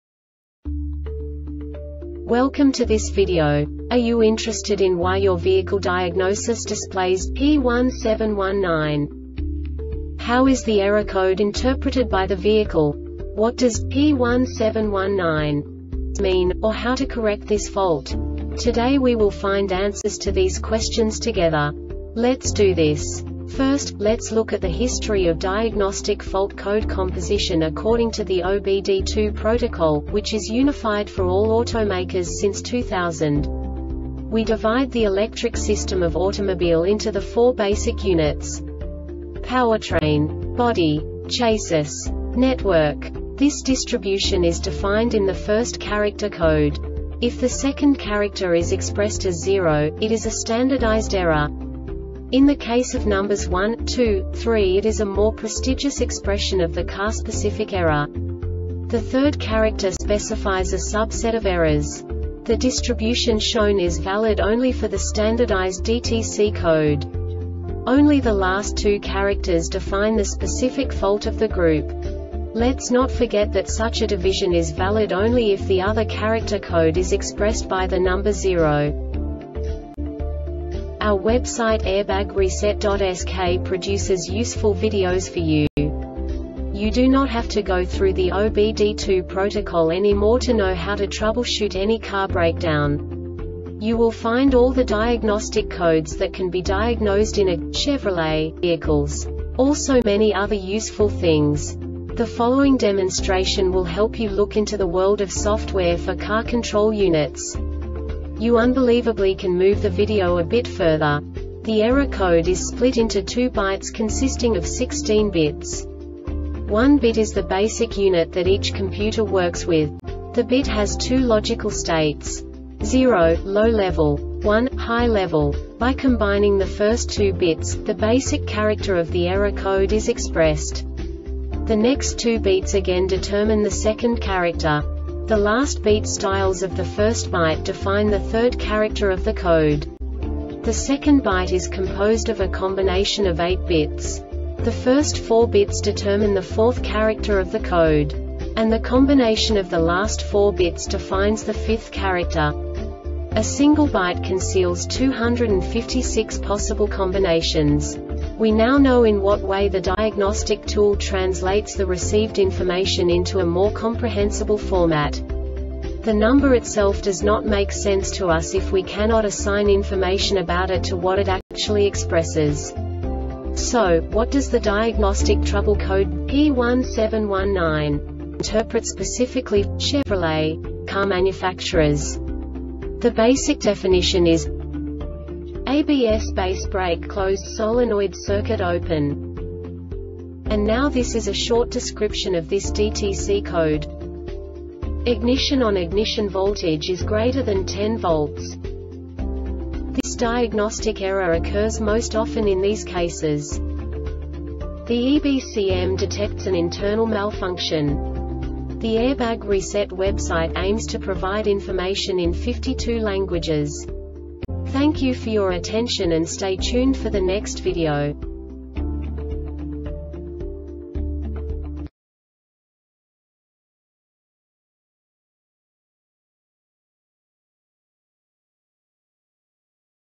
DTC Chevrolet P1719 Short Explanation
Contents:
0:21 Basic DTC analysis according to OBD2 protocol standard.
1:48 Insight into programming
2:58 A diagnostically understandable description of the Diagnostic Trouble Code
3:05 Basic error definition

Make:
Chevrolet
Code:
P1719
Definition:
ABS Base Brake Closed Solenoid Circuit Open
Description:
Ignition ON.Ignition voltage is greater than 10 volts.